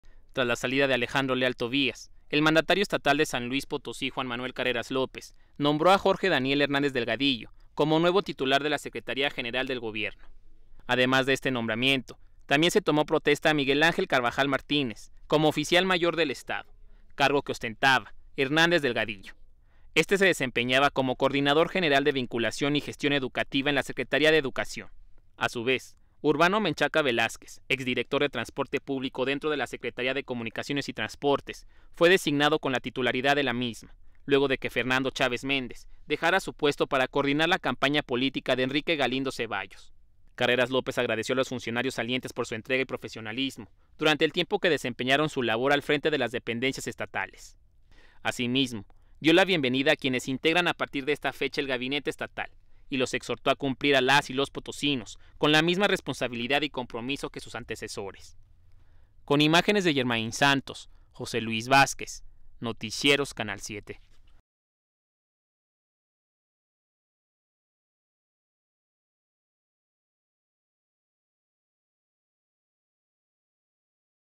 Nombran a Jorge Daniel Hernández Delgadillo titular de la Secretaría General del Gobierno del Estado
SLP.- Tras la salida de Alejandro Leal Tovías, el mandatario estatal de San Luis Potosí, Juan Manuel Carreras López, nombró a Jorge Daniel Hernández Delgadillo como nuevo titular de la Secretaría General del Gobierno.

Además de este nombramiento, también se tomó protesta a Miguel Ángel Carvajal Martínez como Oficial Mayor del Estado, cargo que ostentaba Hernández Delgadillo; este se desempeñaba como coordinador general de vinculación y gestión educativa en la SEGE.

A su vez, urbano Menchaca Velázquez ex director de transporte público dentro de la Secretaría de Comunicaciones y Transportes fue designado con la titularidad de la misma, luego de que Fernando Chávez Méndez dejará su puesto para coordinar la campaña política de Enrique Galindo Ceballos.

Carreras López agradeció a los funcionarios salientes por su entrega y profesionalismo durante el tiempo que desempeñan su labor al frente de las dependencias estatales.

Asimismo dio la bienvenida quienes integran a partir de esta fecha del gabinete Estatal y los exhortó a cumplir alas y los potosinos con la misma responsabilidad y compromiso que sus antecesores.